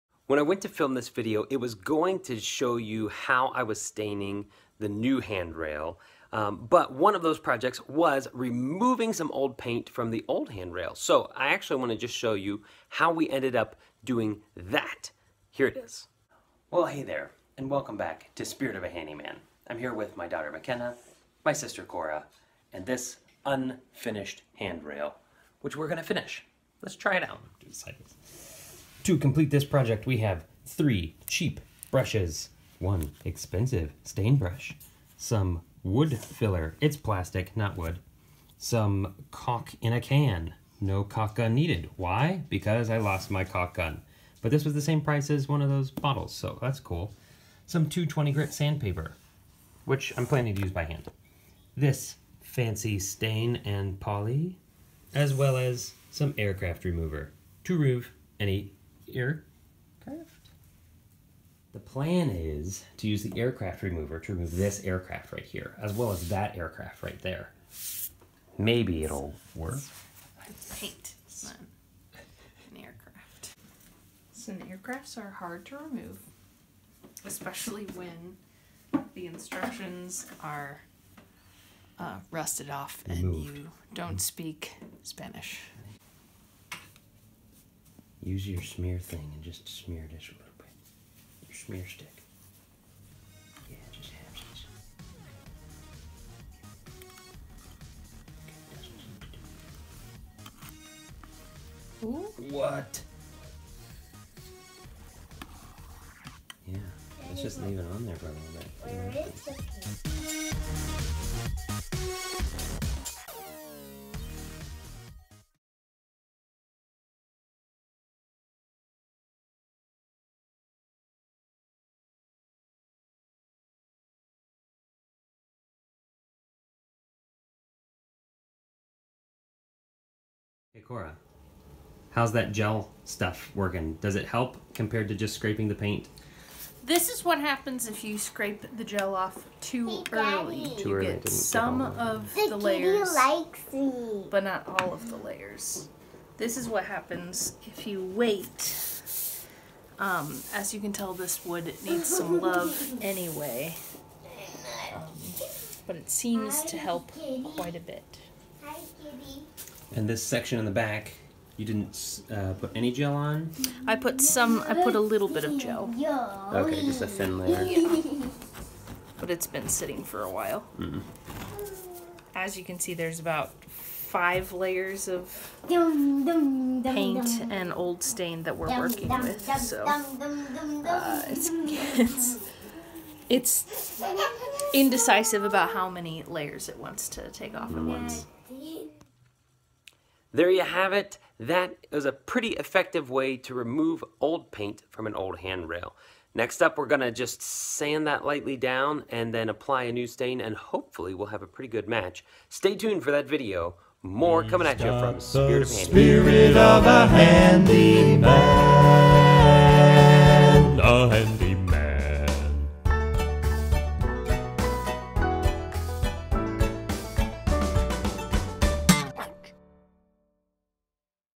Removing Paint From An Old Banister | Paint Stripper To The Rescue
We had a bit of a learning curve to this process because the directions had been eaten away from a previous use with the paint stripper, but I am here to say that this stuff is way better than trying to sand away the old paint. Apply liberally and then scrape off after several minutes. The old layer(s) of paint will scrape off easily! I'm pretty impressed.